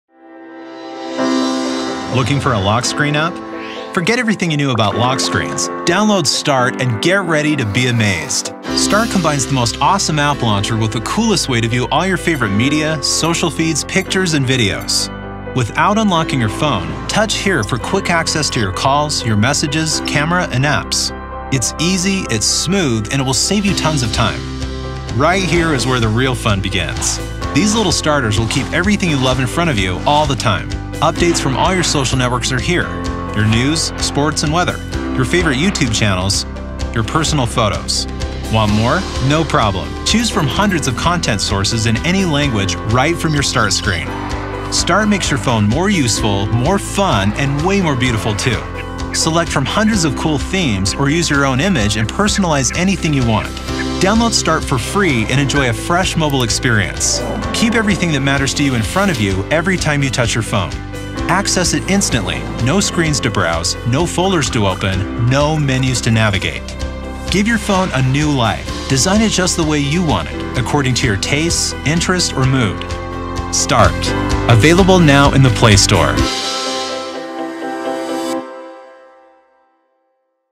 Start - your personalized homescreen
Start having fun with your lockscreen and make it into your Startscreen!

With Start, you get everything you care about the most on your Startscreen. No more browsing through screens, menus to navigate or folders to search … everything you want is available for you on the first

Millions of users enjoy Start. Join the Start revolution – download it on Google play now